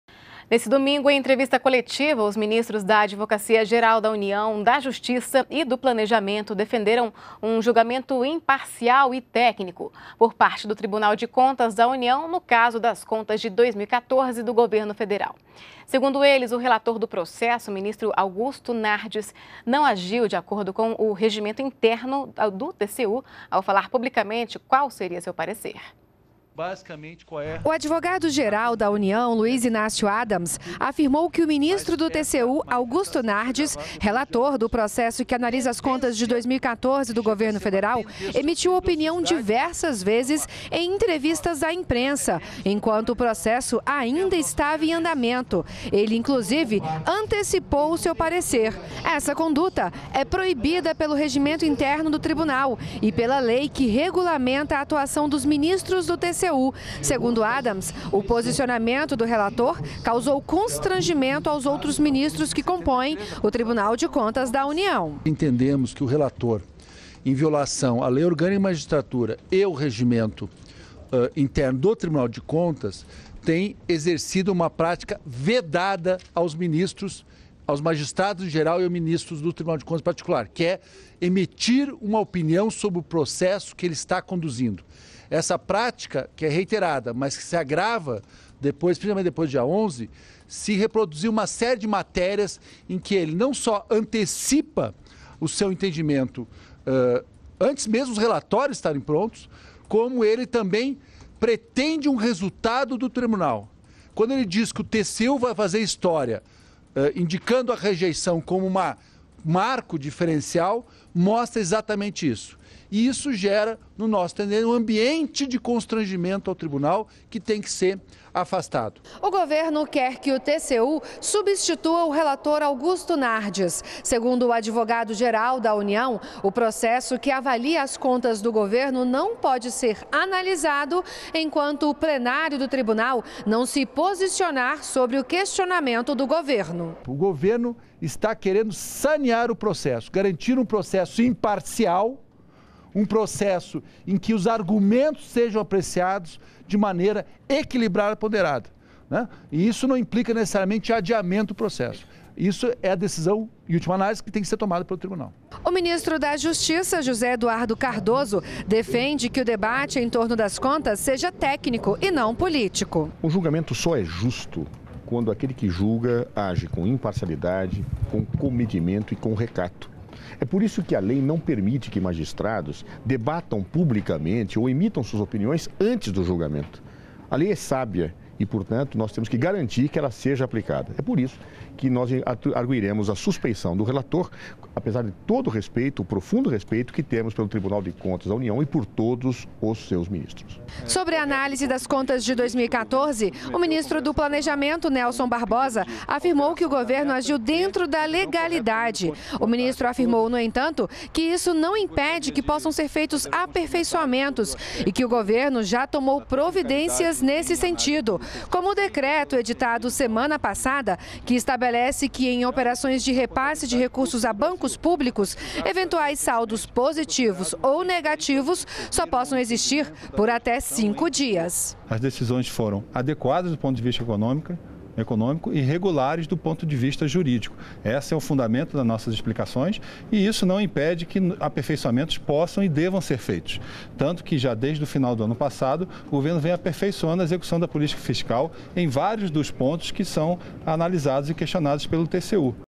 Ministros defendem julgamento imparcial e técnico das contas de 2014 do governo
NBR NOTÍCIAS - 05.10.15: Em entrevista coletiva neste domingo (4), os ministros da Advocacia-Geral da União, da Justiça e do Planejamento defenderam um julgamento imparcial e técnico por parte do Tribunal de Contas da União em relação às contas de 2014 do governo federal. Segundo eles, o relator do processo, ministro Augusto Nardes, antecipou publicamente o seu parecer, conduta proibida pelo regimento interno do TCU.